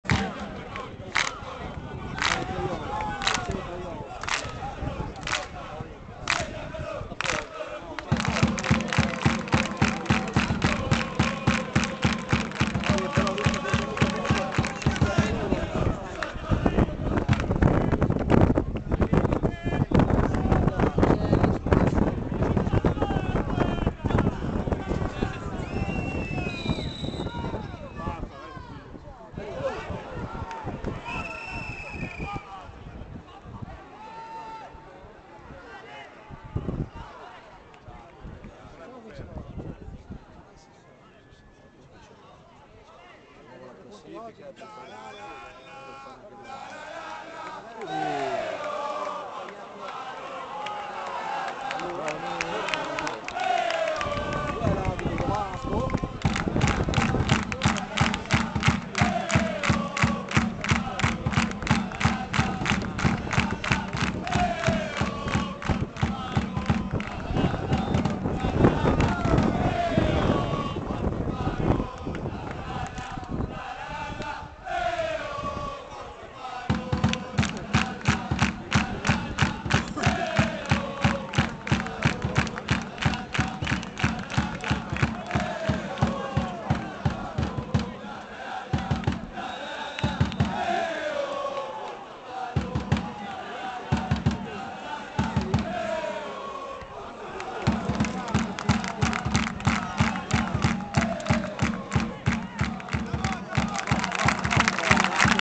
Alma Juventus Fano - San Marino: 2 - 2
Il Fano regala due gol al San Marino ed il risultato si ribalta. Nonostante tutto, sul finire del primo tempo, i granata tentano di riprendere le redini del gioco...